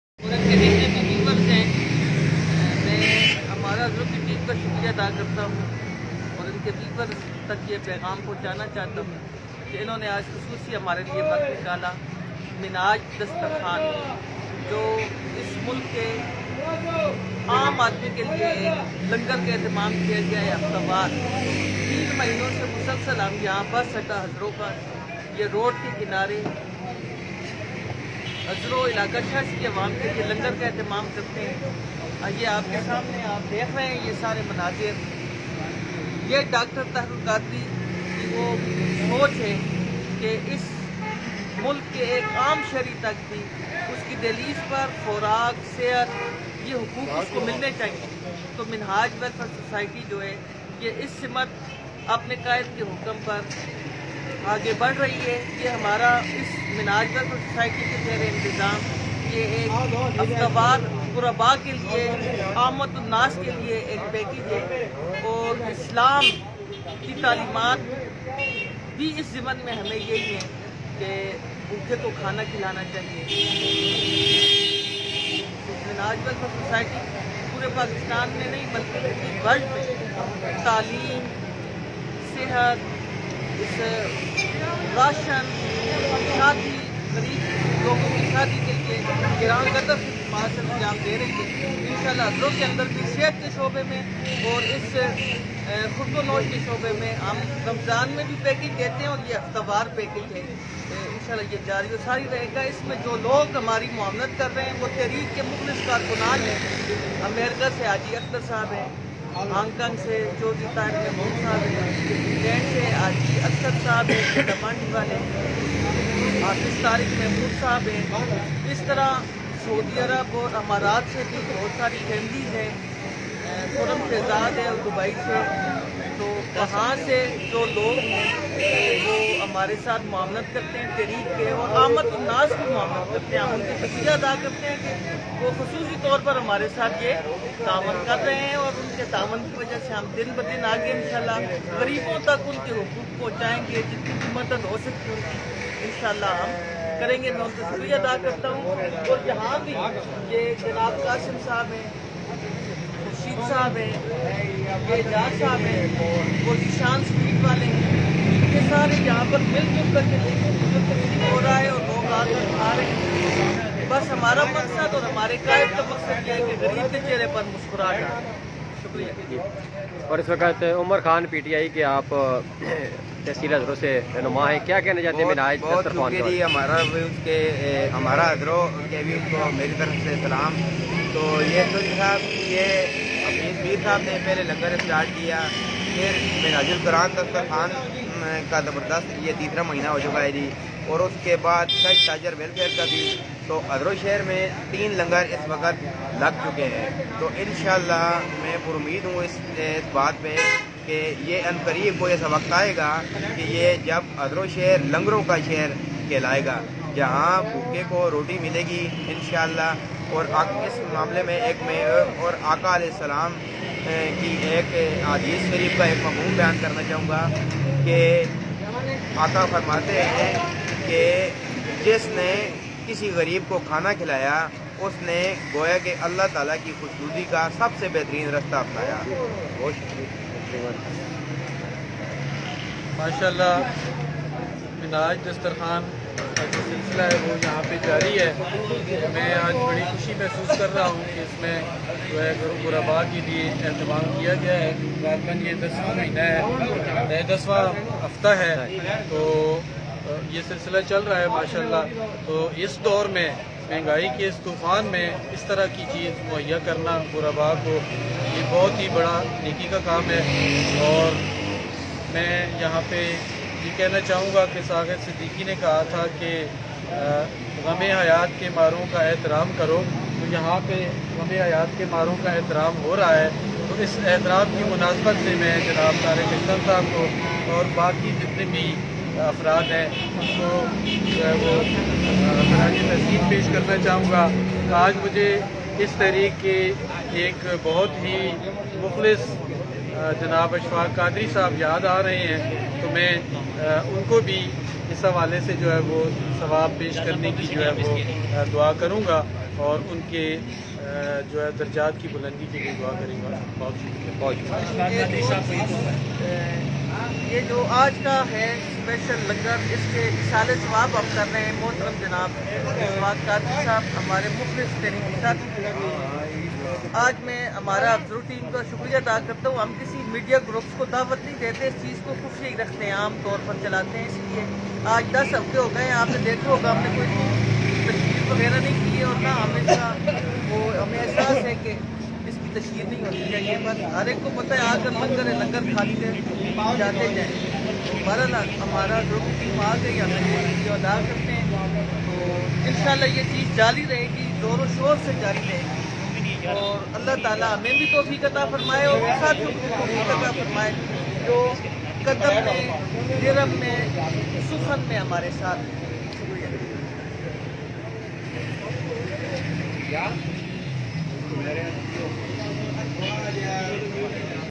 Pakistan Church Village Hazro free lunch Great 👨 2020
منہاج ویلفیئر فاؤنڈیشن دستر خوان حضرو میں گزشتہ 3 ماہ سے لاری اڈہ نزد منہاج سکول میں جاری ھے اس موقع پر طارق عشتم صاحب ، صفدر بھٹی صاحب اور میں عمر خان نے گفتگو کی بہت شکریہ Hamara Hazro فیس بک پیج ۔۔۔۔۔۔۔۔۔۔۔۔۔///